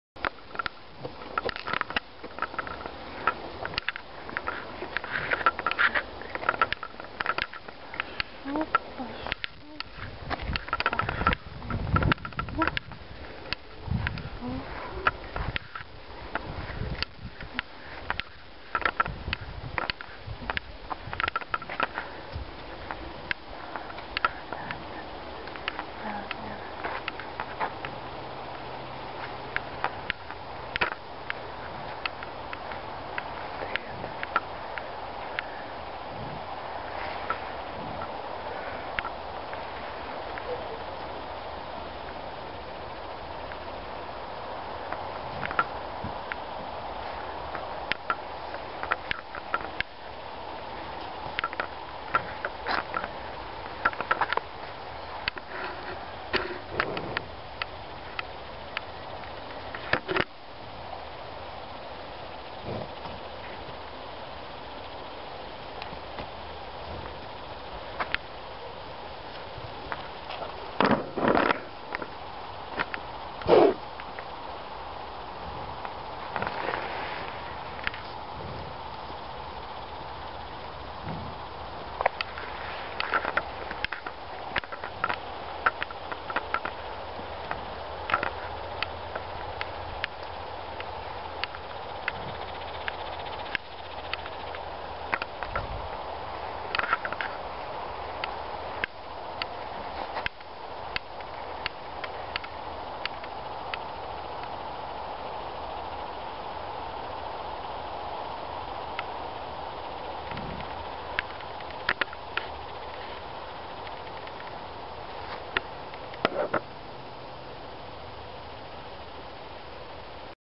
Bisons in Toksovo
Video made ​​by 08:14:2012

These unique animals there are only four. Without selection, they are dying ... They also have no owner, always short of food, you need to bring teretorii their stay at the order and build a new winter home for them. Help preserve these unique bison and raise their livestock in the national park Toksovo.All those who can help them ... Help please!